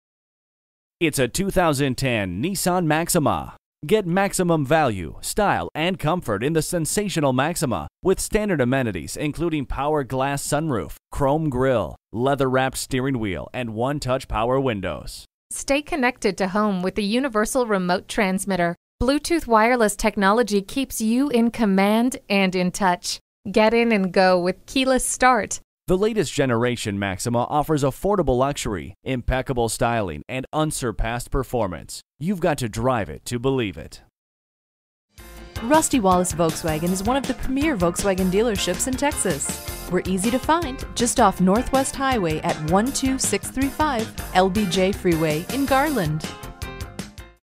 2010 Nissan Maxima Dallas TX Garland, TX P5826H - SOLD
Year: 2010
Make: Nissan
Model: Maxima
Trim: Continuously Variable Automatic
Engine: 6 Cylinder
Transmission: Continuously Variable Automatic
Color: Ocean Gray
Interior: Charcoal
Mileage: 44423
Stock #: P5826H

Address:
12635 LBJ Freeway